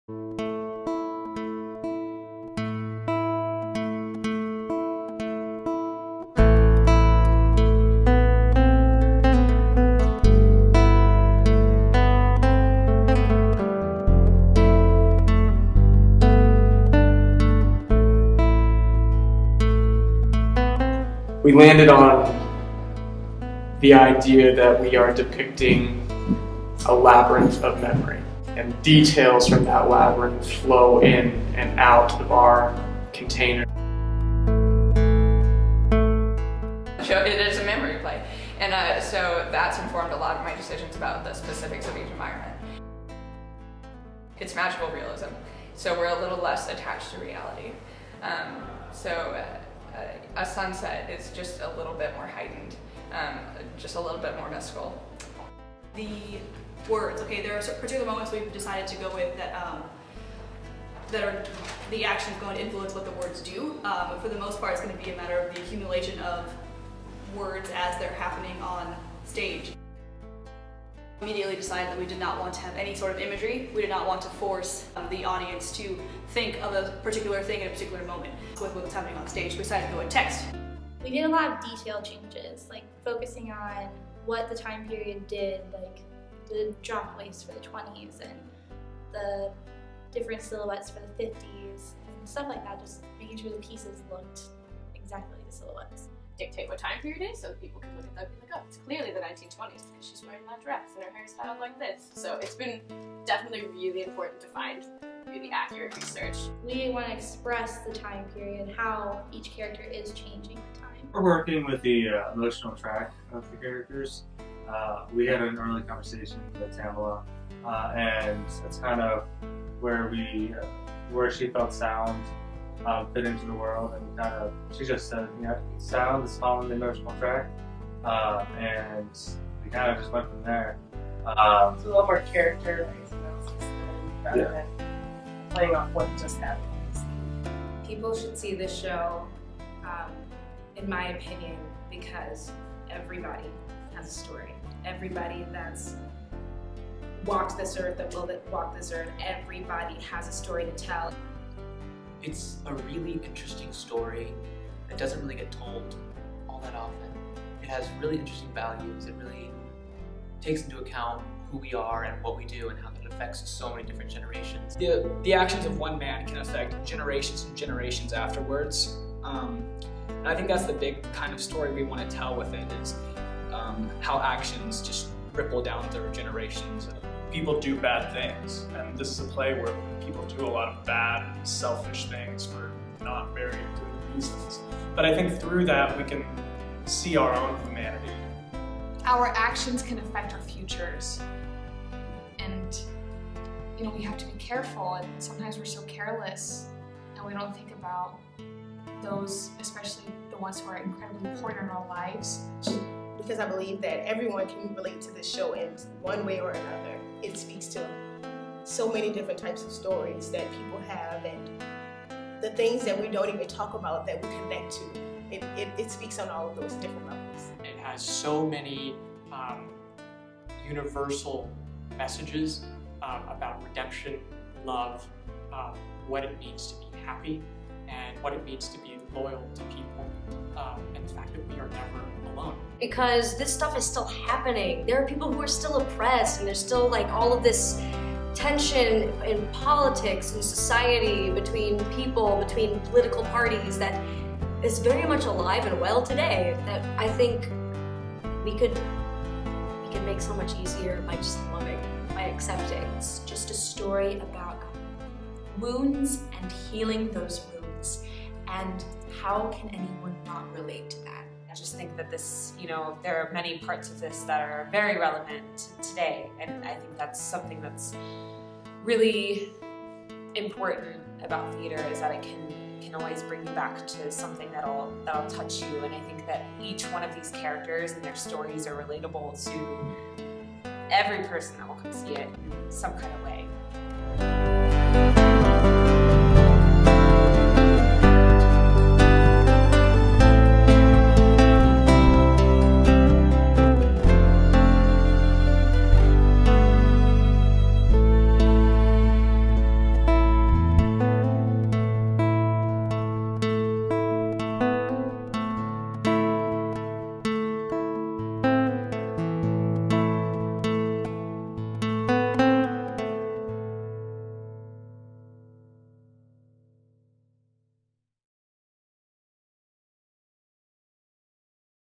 Webster Conservatory: The House of the Spirits
The House of the Spirits
by Caridad Svich
Based on the novel by Isabel Allende
Directed by Tamilla Woodard
Feb. 19-23 & Feb. 26-Mar. 2
7:30pm, Sundays at 2:00pm
Emerson Studio Theatre
For Reservations Call
Please note, this show is intended for mature audiences due to strong violence and language.
Promotional Video by Alex Glow
Organized by Brian Coats